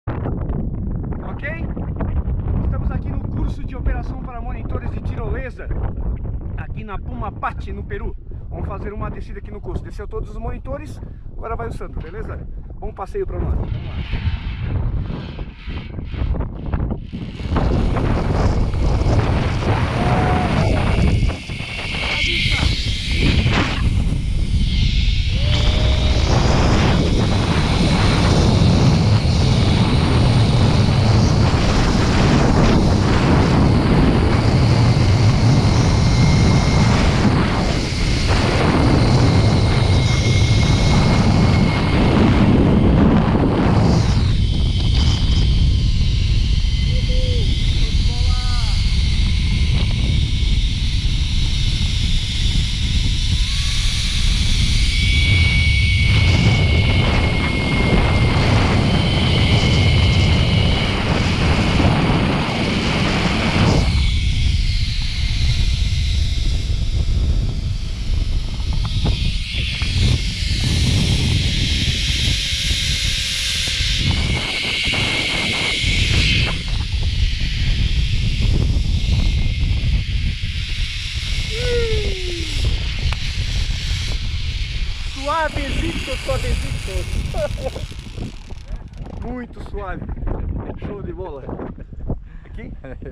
Puma Path Zip Line em Ollantaytambo.
PRIMEIRA ZIP LINE NO VALE SAGRADO EM OLLANTAYTAMBO PERU. 3.200 metros de altitude. Quase 2 km de Zip Line com uma vista privilegiada. Mais informações Um projeto de Tarapoto Adventure e Projeto Aventura Tirolesas do Brasil. Com os brother Eddy Wilfredo Gibaja, Scott Masciarelli, Waldemar Barrientos, Percio Grande